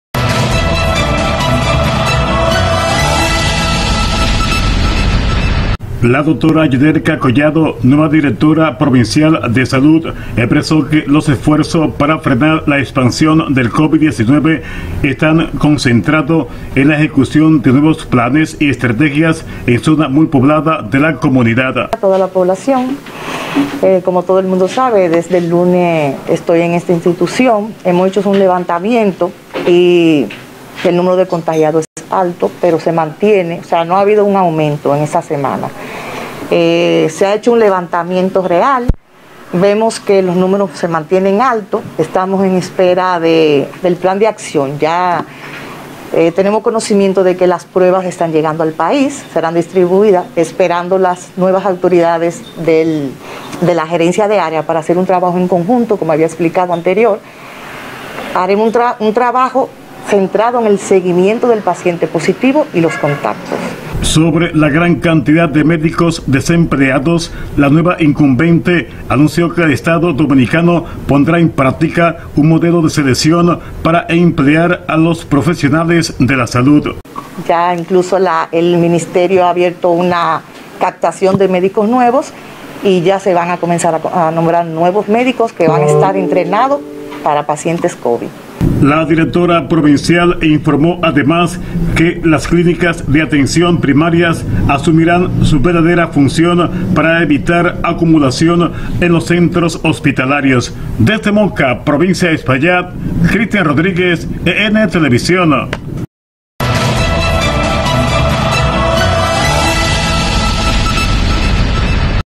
Yudelka Collado, directora provincial de salud,+ anuncio que su prioridad es COVID-19 en Espaillat
Periodista: Corpus Cristian Rodriguez